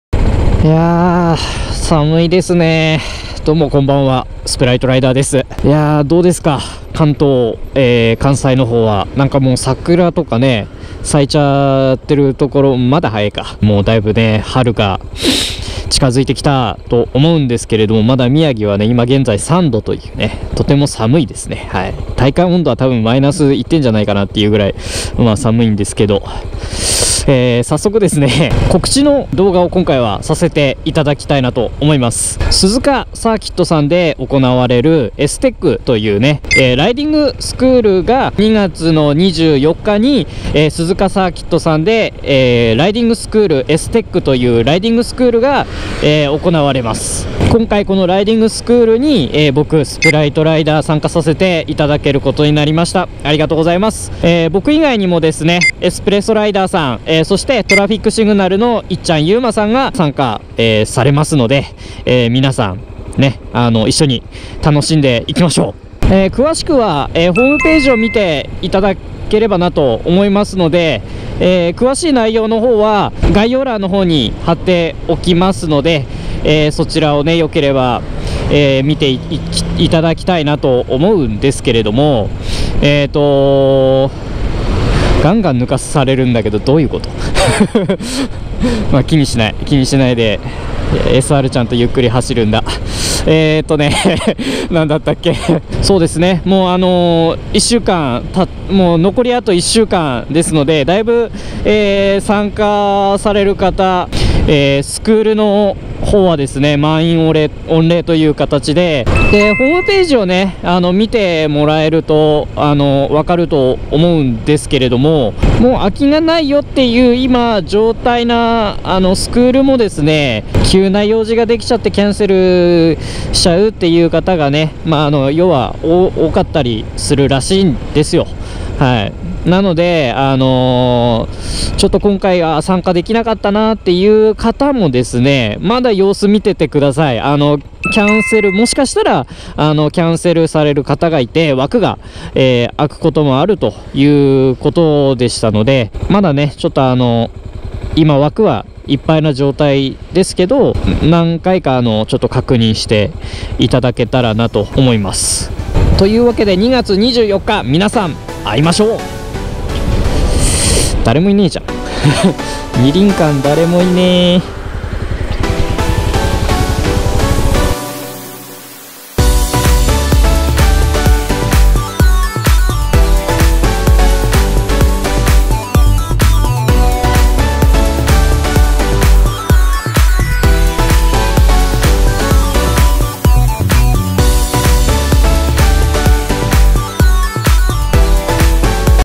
【イベント告知動画！！】2/24鈴鹿サーキットで開催されるライディングスクールSTEC BikeOnlyDayに参加決定！！
今回は2月24日に鈴鹿サーキットで開催されます【STEC BikeOnlyDay】に参加することが決まりましたので簡単にではありますが告知動画を作らせて頂きました！！

ライディングスクールではレンタルバイクが用意されています！！
プロテクターも借りれるので安心してライディングスクールを受けることができます！！